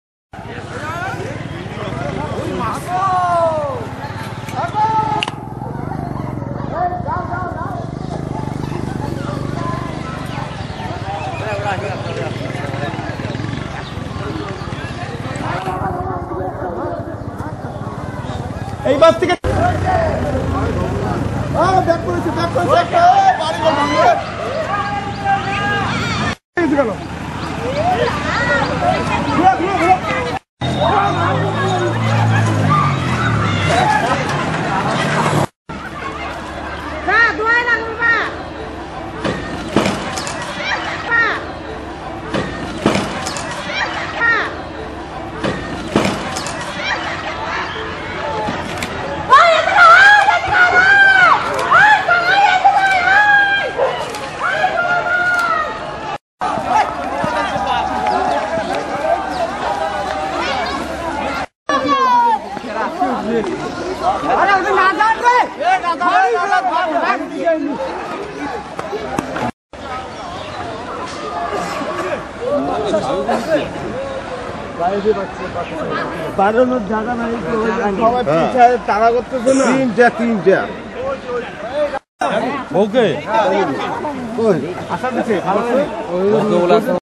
ELEPHANT TERROR DHUPGURI
Elephant Entered Dhupguri Town First time